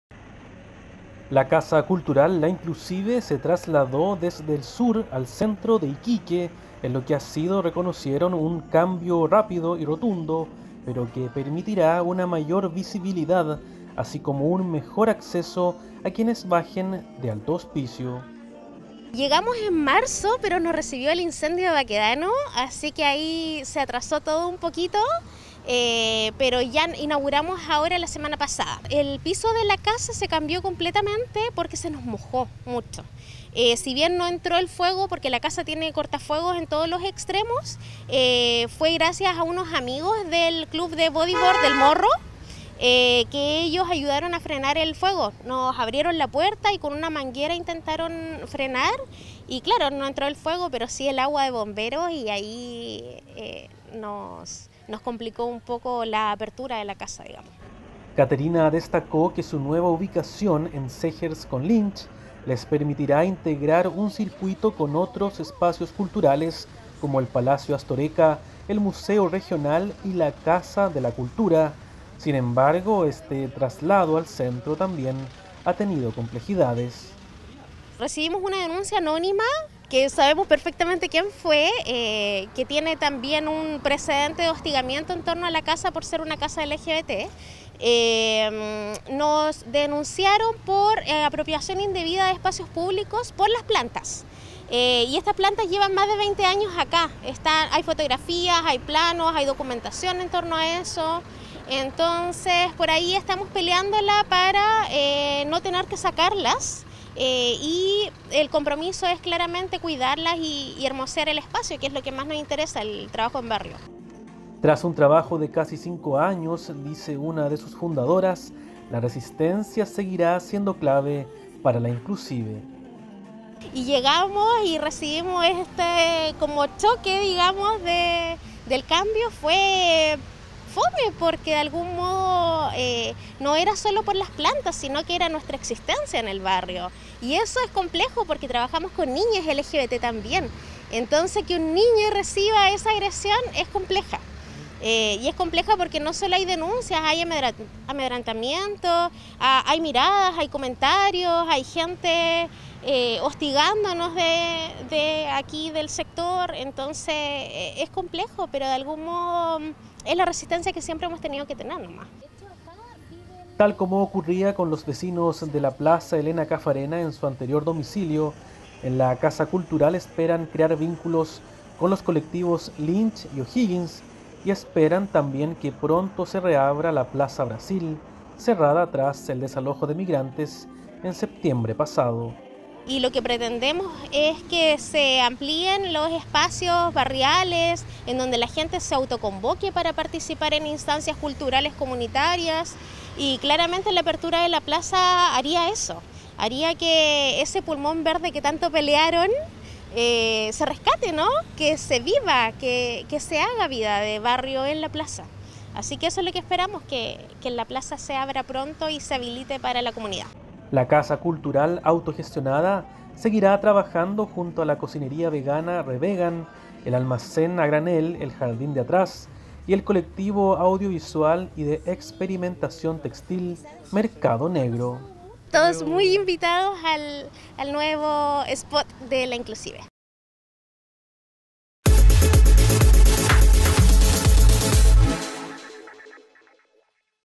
Los desafíos de “La inclusive” en su nueva casa en el centro (06.05.2022) Iquique TV
La casa cultural inauguró el viernes pasado su nueva dirección en Lynch con Zegers. Junto con destacar la cercanía a otros espacios culturales de Paseo Baquedano, una de sus fundadoras también reconoció que han sido víctimas de hostigamientos al reconocerse como un lugar de encuentro LGBT.